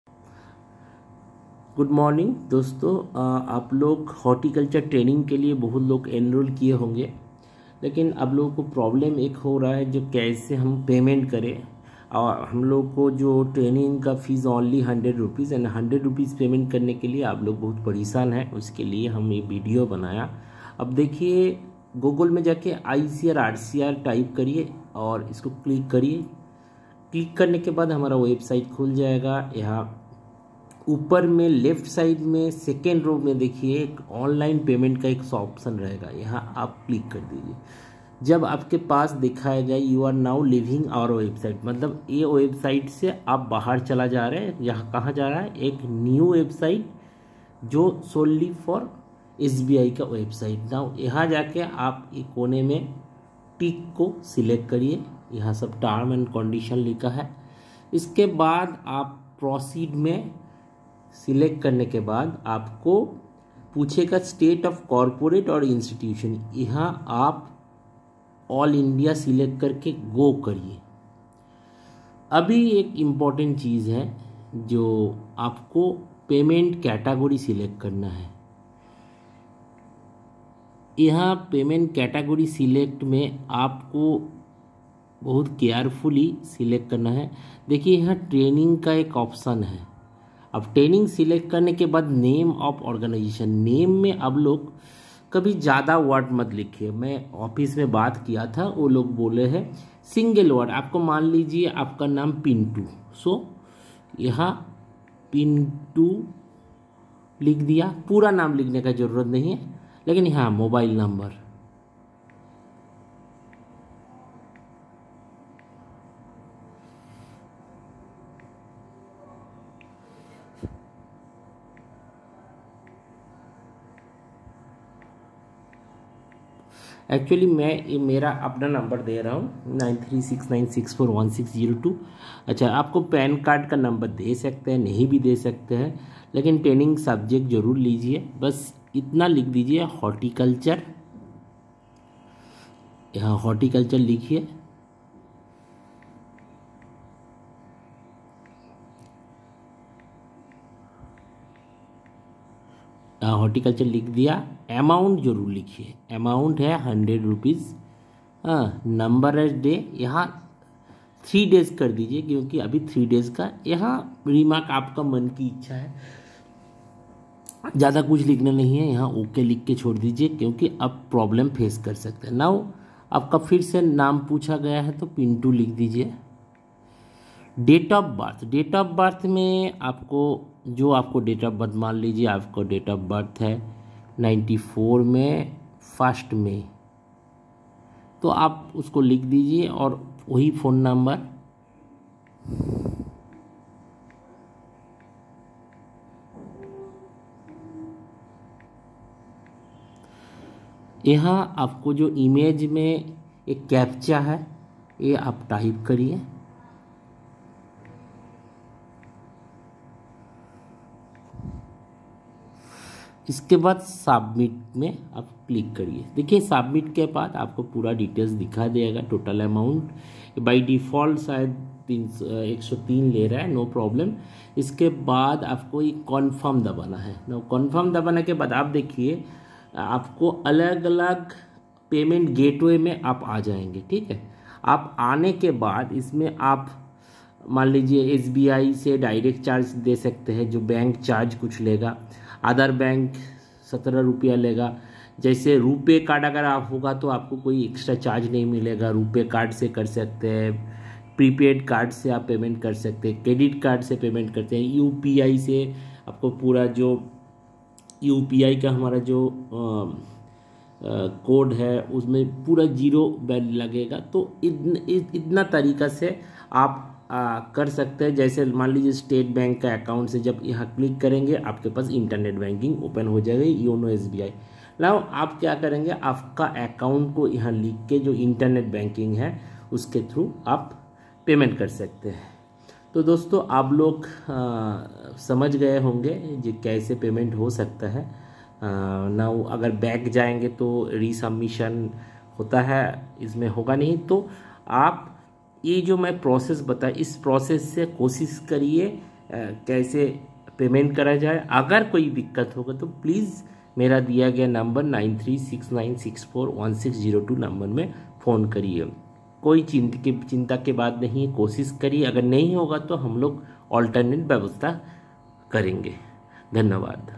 Horticulture Payment through SBI collect
Here I describe how you can pay 100 rs through SBI collect. method is simple and safe.